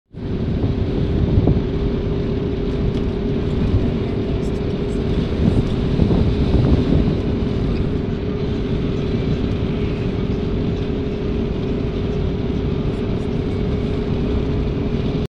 vista de cerros tiawanako bolivia
Vista desde la carretera de cerros en tiawanako bolivia